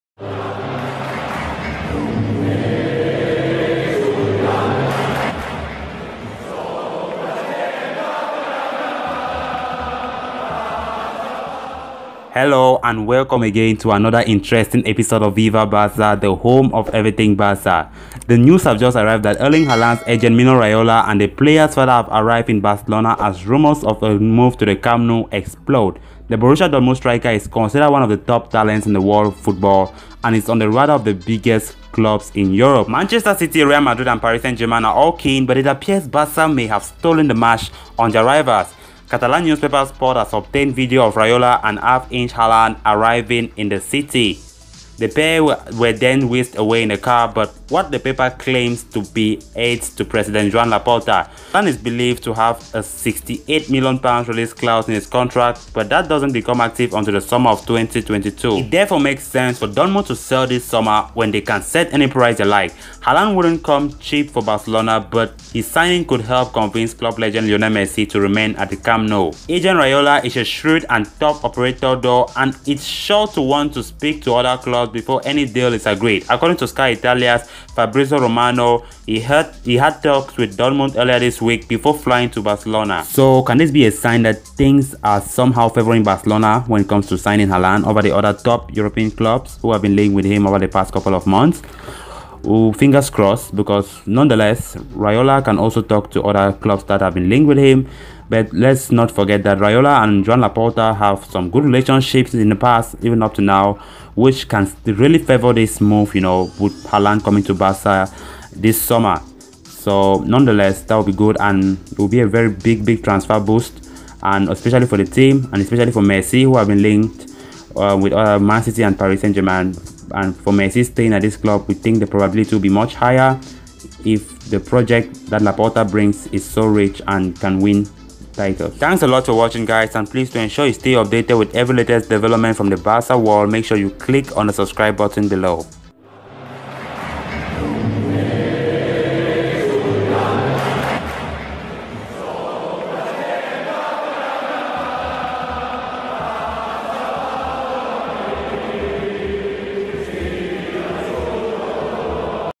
Wow!!! Erling Haaland's agent Raiola and his Dad land in Barcelona for transfer talks
ERLING HAALAND'S FATHER ALF-INGE AND AGENT MINO RAIOLA LAND IN BARCELONA 'FOR TALKS OVER £154M MOVE'
Months of speculation over a potential big-money move for Erling Haaland received fresh fuel on Thursday after the striker's father, Alf-Inge, and his agent, Mino Raiola, were pictured arriving in Barcelona, before reportedly travelling to Madrid. The prodigious Borussia Dortmund star has been repeatedly linked with a transfer to the Camp Nou, along with numerous other top clubs.